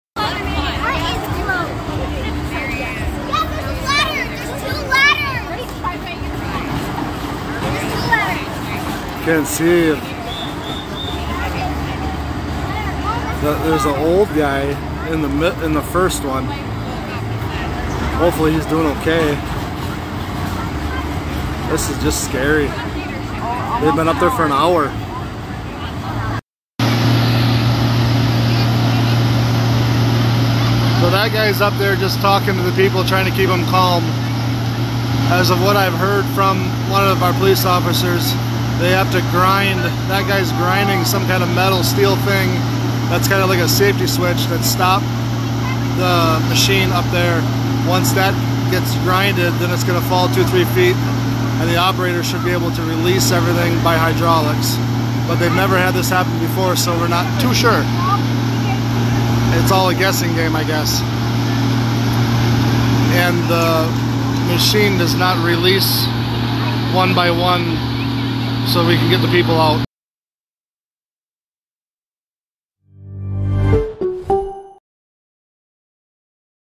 Fair-goers get trapped upside-down on a ride at the Forest County Festival in Crandon, Wisconsin
A fear for many was unlocked Sunday at the Forest County Festival in Crandon, as a carnival ride got stuck while riders were upside-down.

Those on the ride were stuck for at least three hours while first responders worked to find a solution.